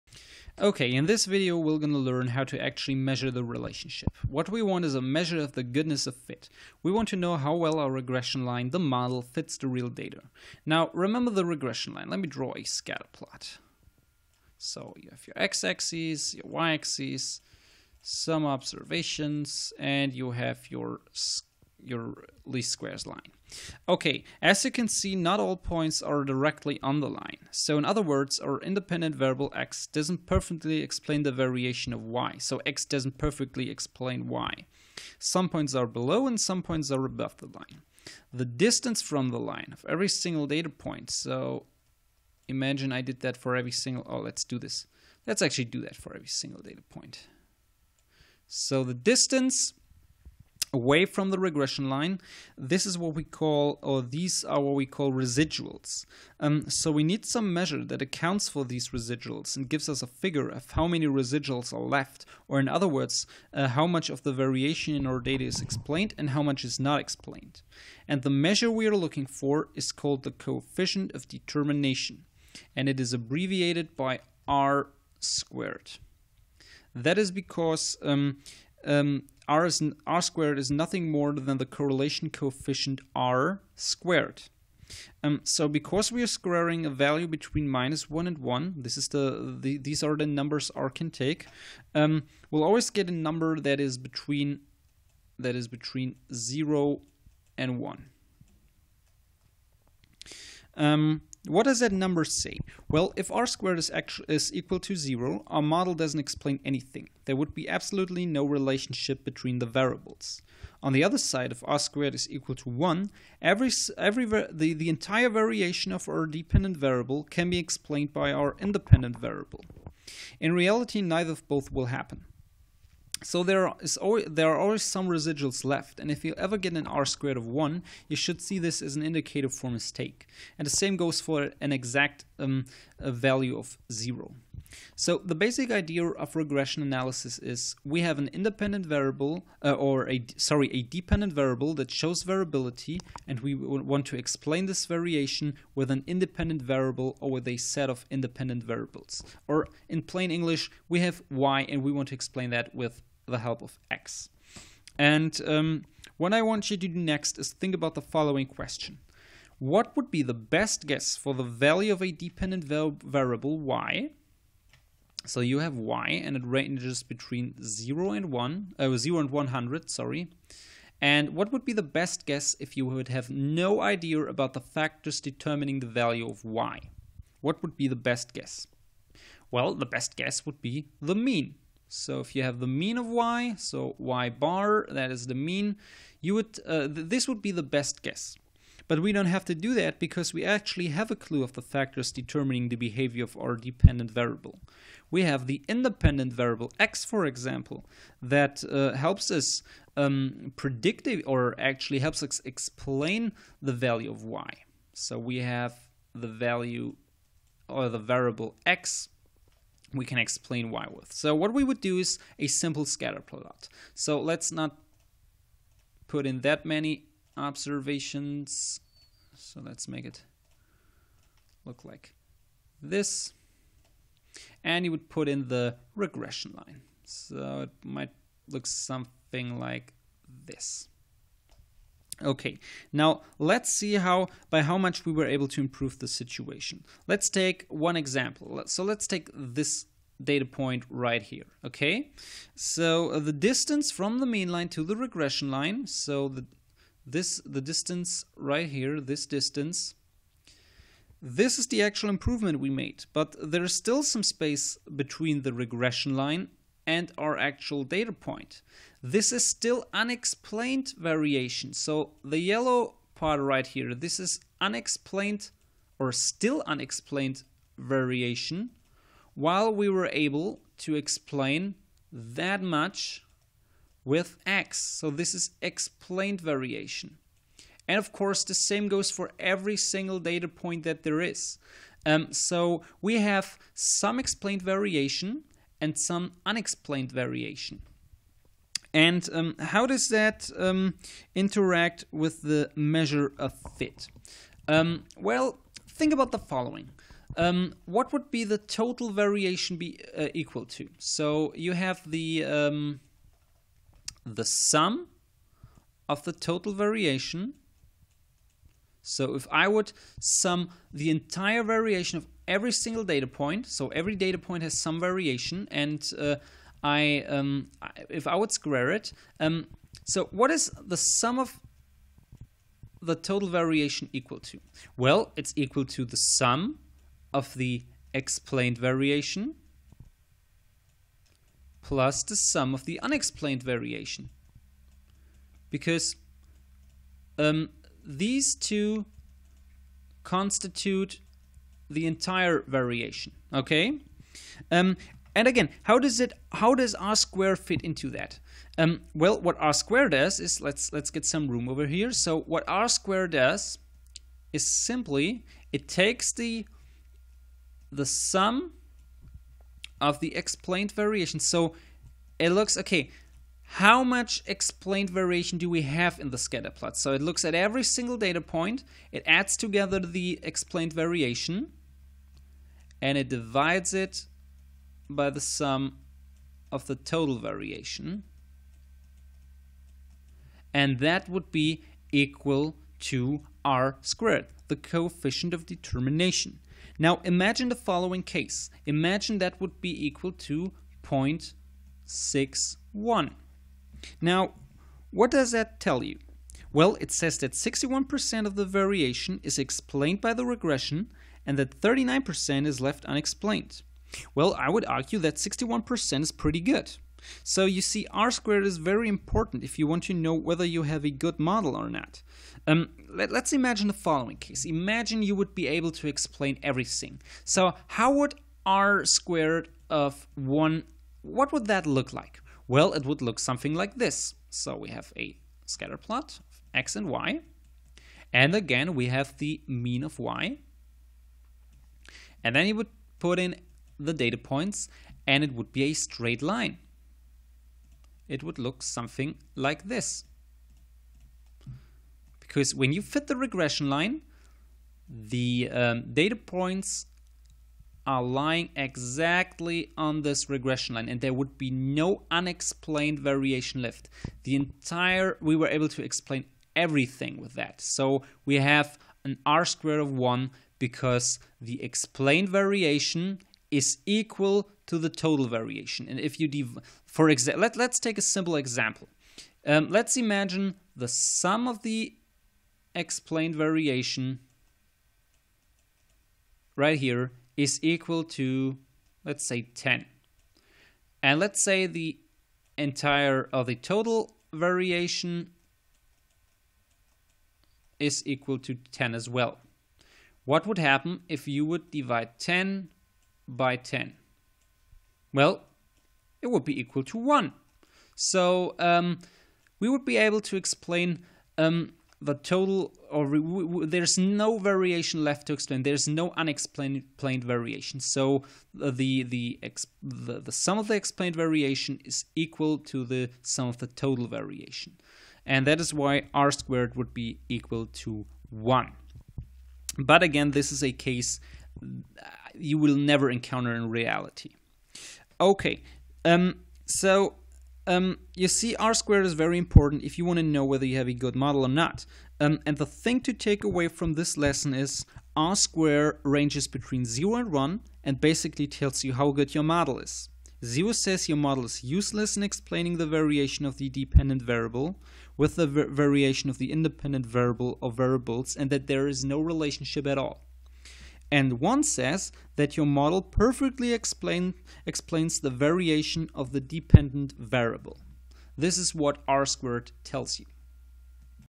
Principles of Cliometrics (Episode 21) - Coefficient of Determination
What is the correlation of determination and what is the idea behind it?